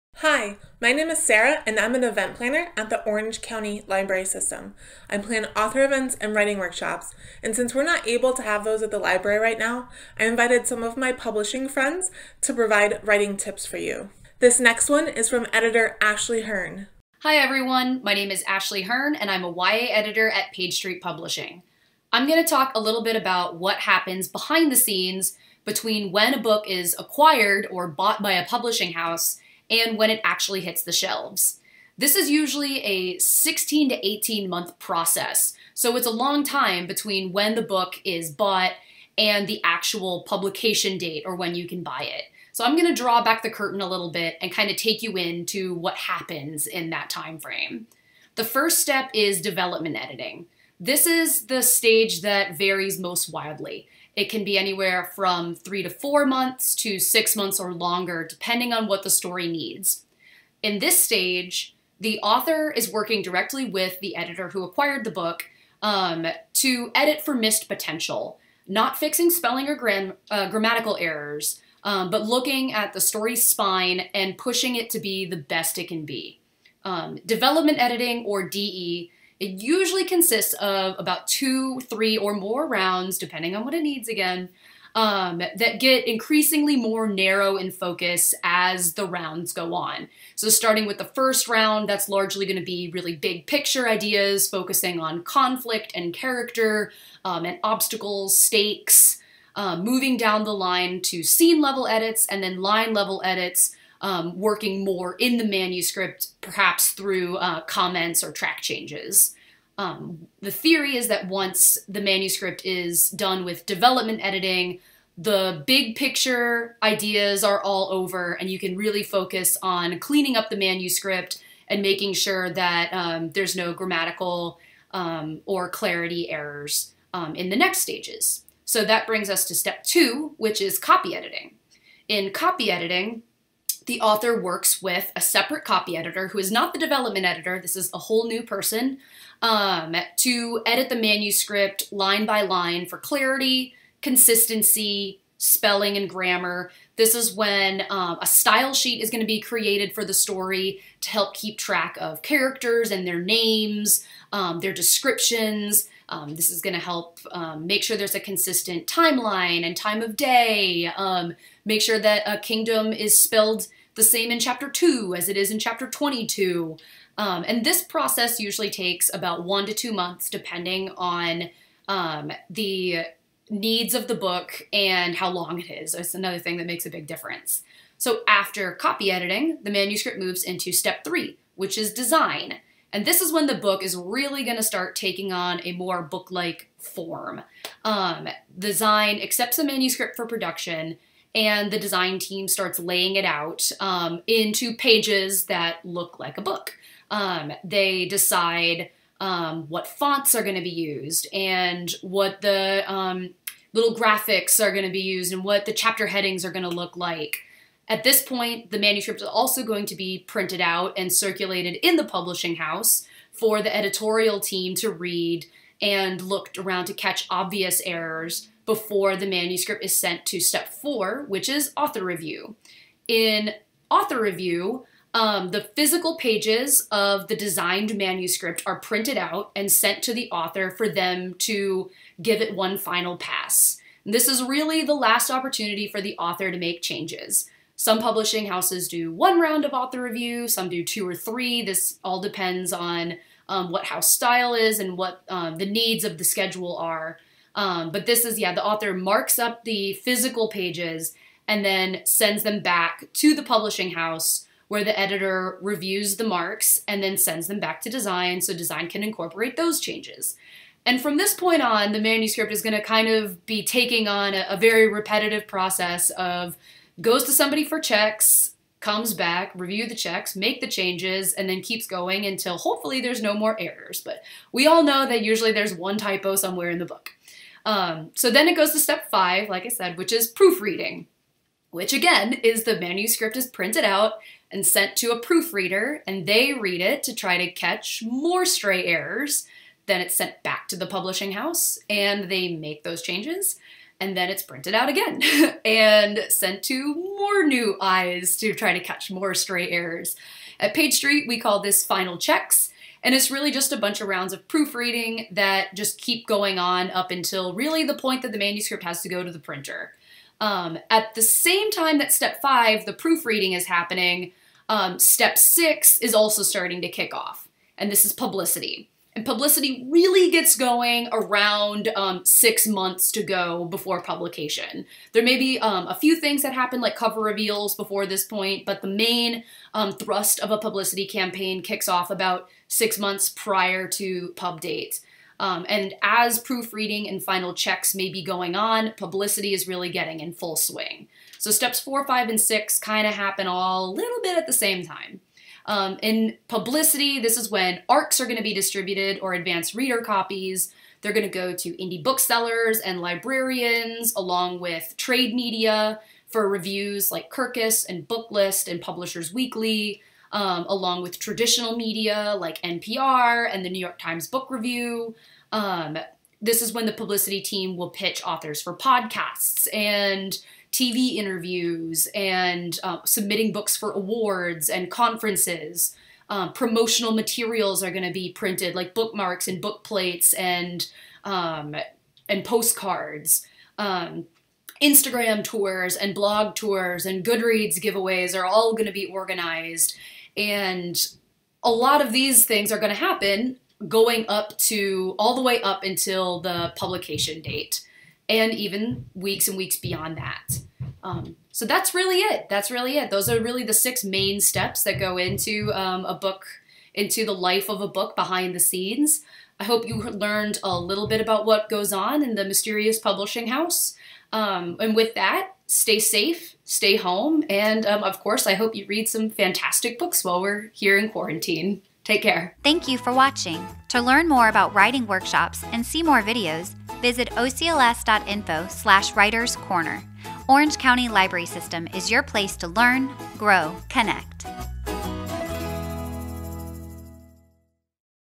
Writing Tip of the Week with Ashley Hearn
Ashley Hearn, editor at Page Street Publishing, walks you through the publication process from after the contract is signed until the book makes its way to bookstore shelves. Learn about the different kinds of editing and promotional efforts that occur behind the scenes in a traditional publishing house.